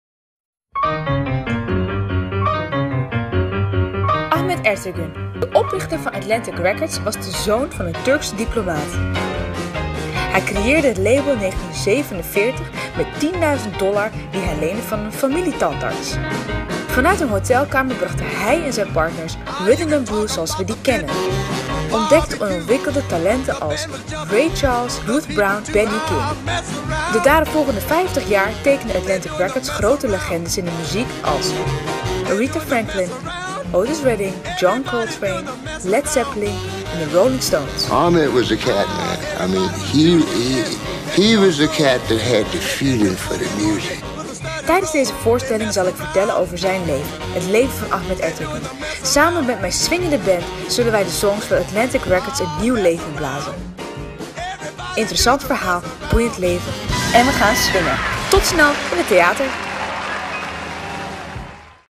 Karsu Plays Atlantic Records
Samen met een ijzersterke band speelt Karsu jazz en blues uit de begintijd van Atlantic Records. Ze vertelt over oprichter Ahmet Ertegün, van Turkse komaf, die één van de grondleggers is geweest van de 'black music' en talenten naar voren schoof als Ray Charles, The Rolling Stones, Aretha Franklin, The Bee Gees, Led Zeppelin, Eric Clapton en Cher.